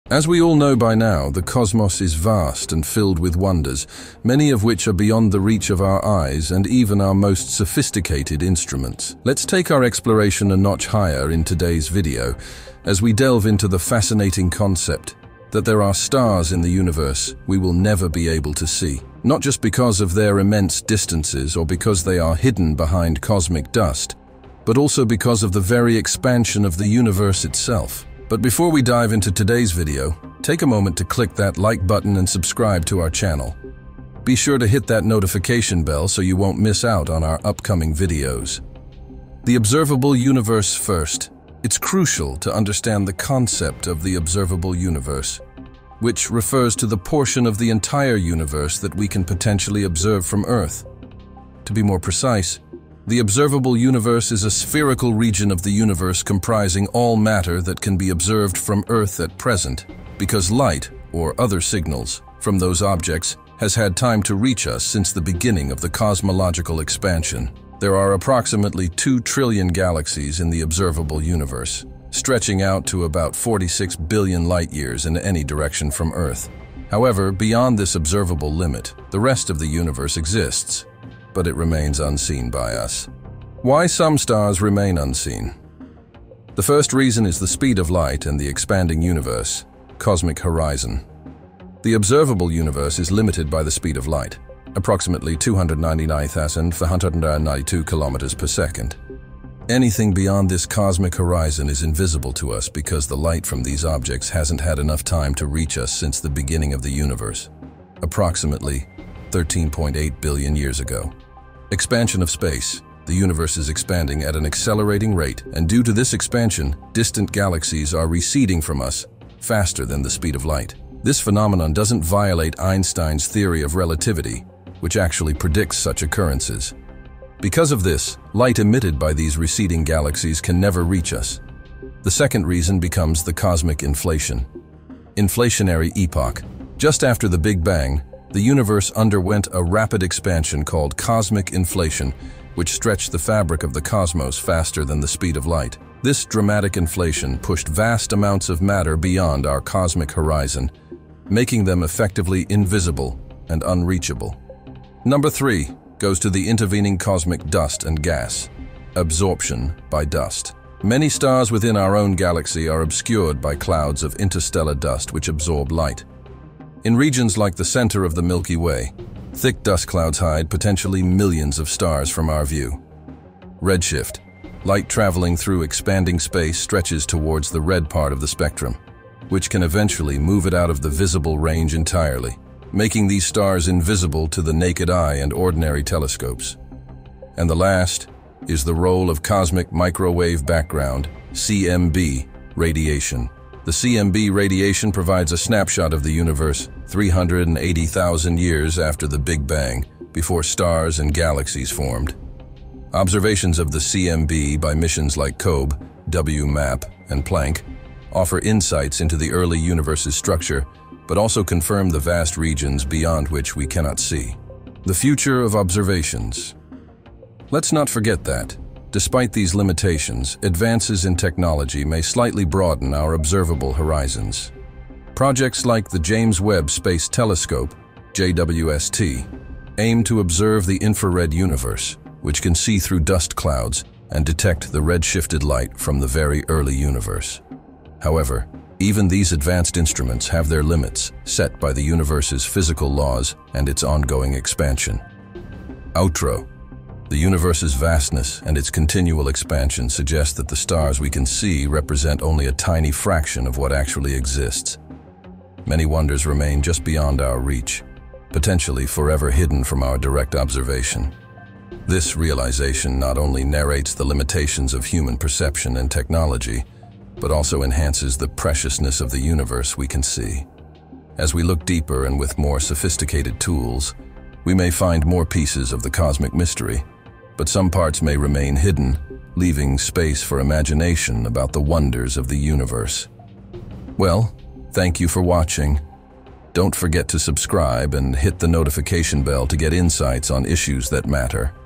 Exploring the Unseen: Stars Beyond Our Reach
A cosmic journey as we delve into the mysterious depths of the universe in today's video. Join us as we uncover the fascinating concept of stars that lie beyond the realm of our observation. From the cosmic horizon to the effects of the expanding universe and cosmic inflation, learn why some stars remain forever unseen.

Discover the role of intervening cosmic dust and gas, as well as the intriguing insights provided by Cosmic Microwave Background radiation. Despite technological advancements, there are still limitations to what we can observe.